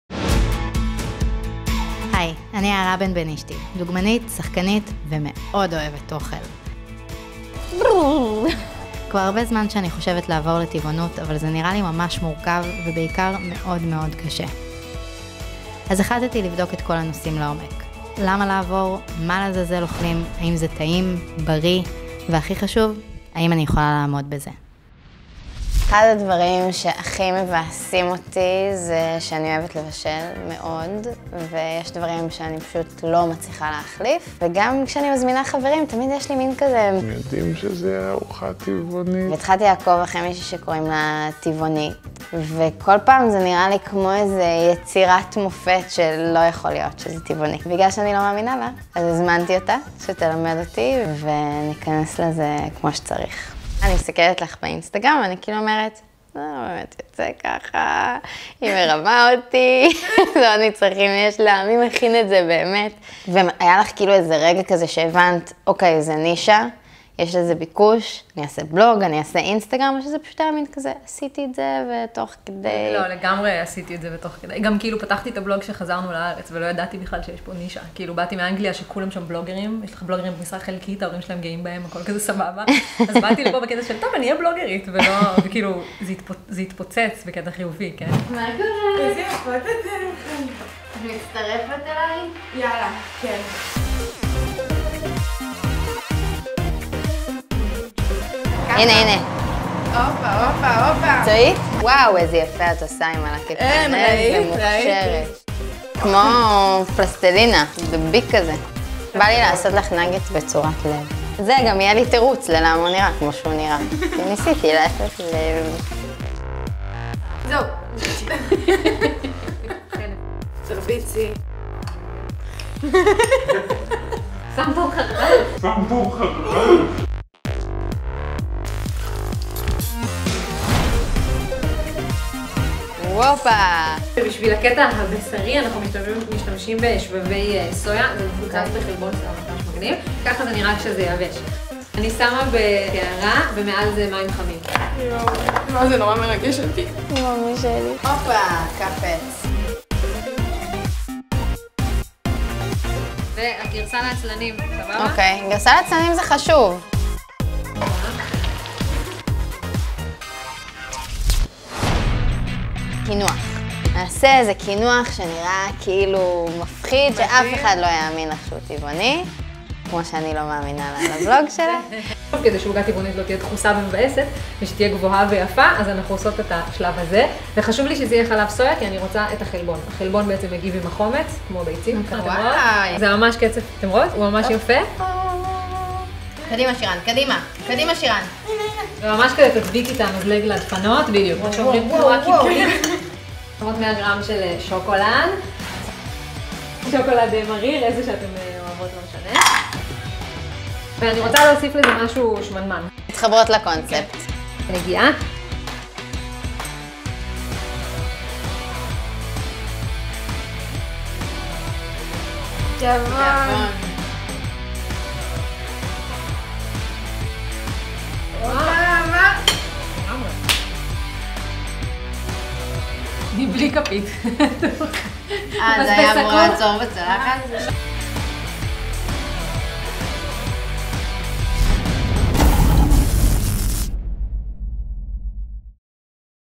יערה בנבנישתי מנסה טבעונות - פרק שלישי
הפרק השלישי בסדרת האינטרנט על יערה בנבנישתי עוברת לטבעונות כבר כאן, והפעם - איך מבשלים בבית ארוחות טבעוניות משגעות וטעימות?🍝🍰🥘

יערה, שחקנית ודוגמנית, פנתה אלינו אחרי תקופה ארוכה במהלכה שקלה לעבור לטבעונות.
יחד איתה, החלטנו לצאת למסע מתועד ומרגש שבו יערה מגלה את כל התשובות לשאלות שמטרידות אותה ועוד רבים אחרים לקראת המעבר לטבעונות.

למה בכלל להפסיק לצרוך מוצרים מן החי? מה לעזעזאל אוכלים? האם אוכל טבעוני יכול להיות טעים? בריא?

מוזמנים לצפות בפרק השלישי מתוך סדרה של פרקים, לגלות יחד עם יערה עולם שלם של אוכל טעים, מושחת ובריא, טבעוני וצבעוני, לתמוך ולתת לה מלא אהבה על האומץ להיחשף וללכת אחרי האמת שלה.

תודה ענקית ל- TivonEat שנרתמה למשימה ושיתפה אותנו במתכונים הטבעוניים המעולים שלה!